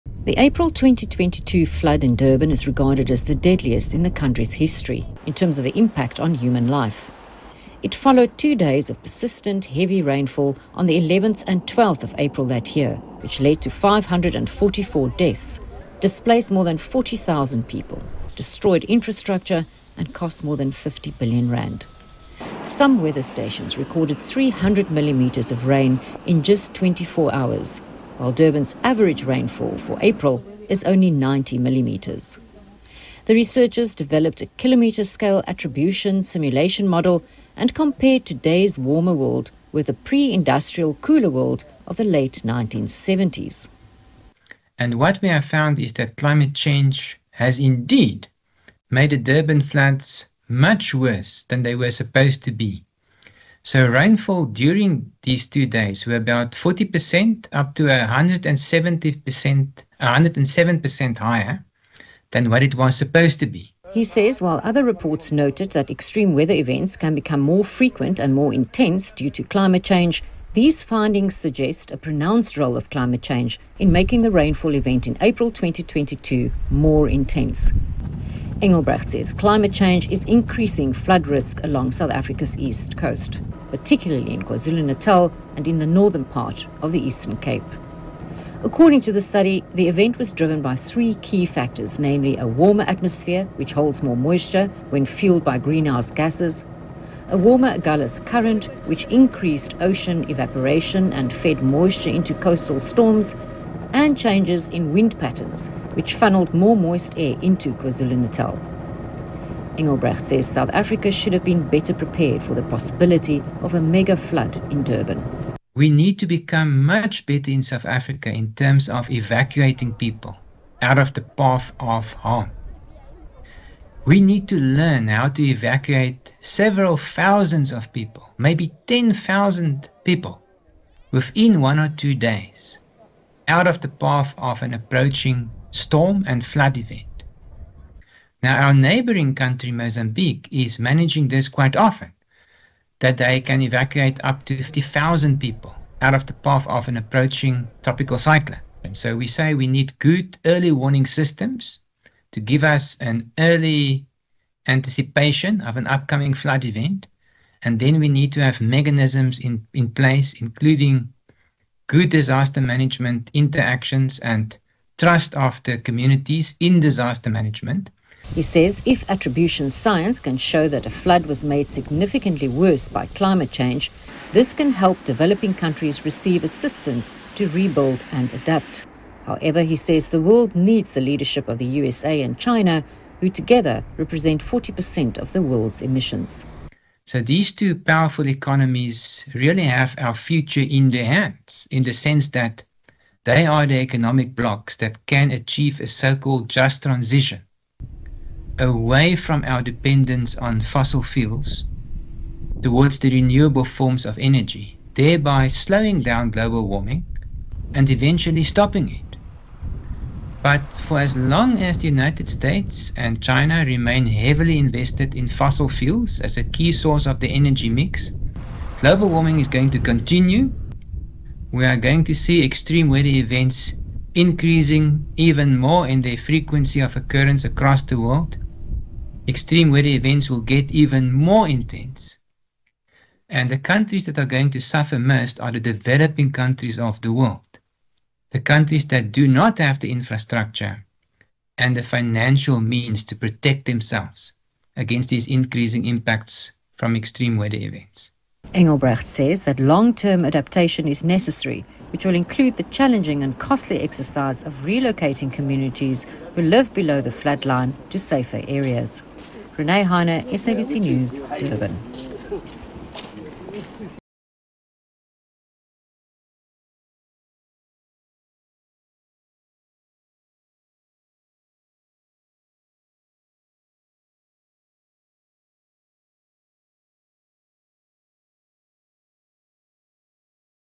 Climate change worsened Durban floods in 2022, research shows
Research has shown that Durban's deadly April flood in 2022, which claimed more than 500 lives, was significantly worsened by climate change.

Scientists at Wits University's Global Change Institute have, over three years, developed a climate modelling attribution system, which can quantify the role of climate change in the flood that caused large-scale destruction in Durban and surrounding areas.

The scientists say the research can be regarded as a major contributor to analysing how global warming is driving extreme weather events.